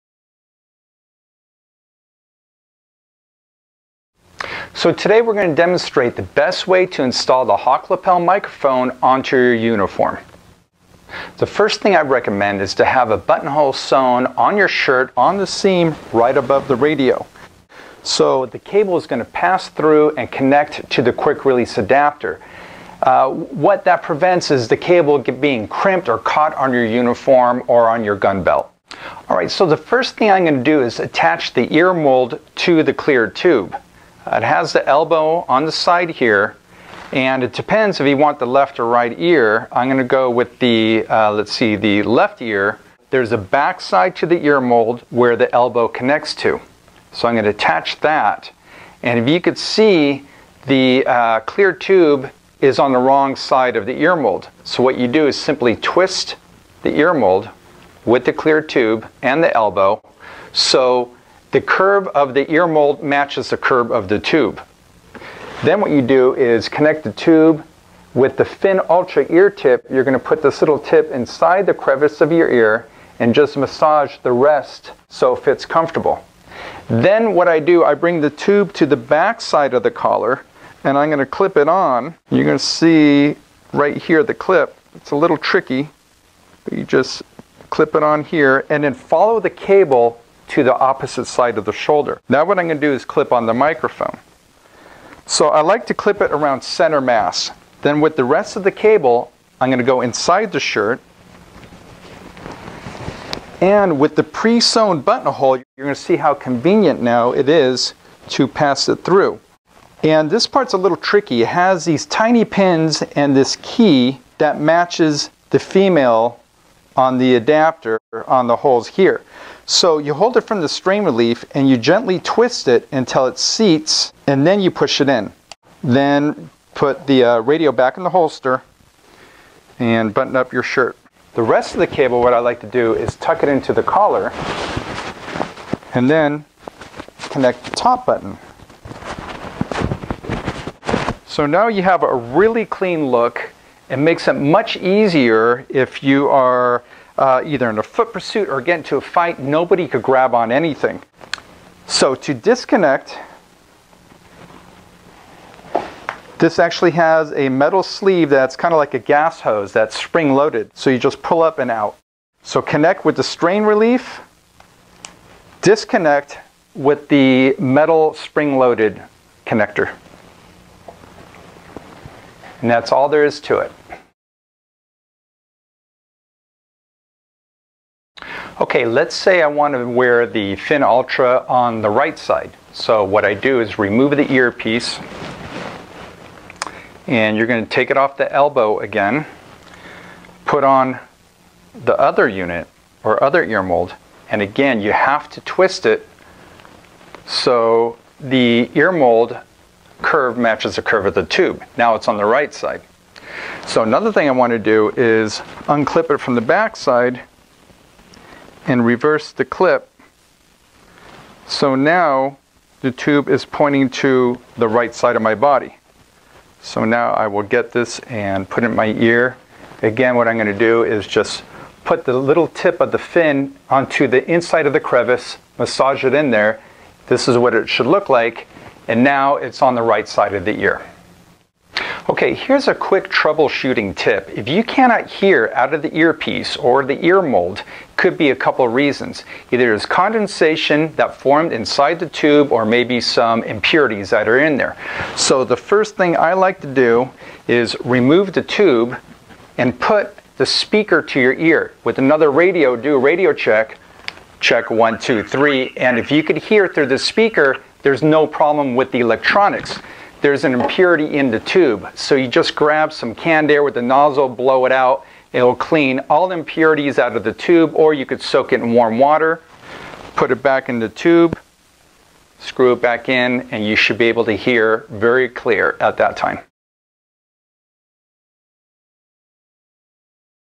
Hawk Lapel Microphone Installation
This video gives step-by-step instructions on the use of Ear Phone Connection's Hawk Lapel Microphone and is helpful for reducing wear and tear.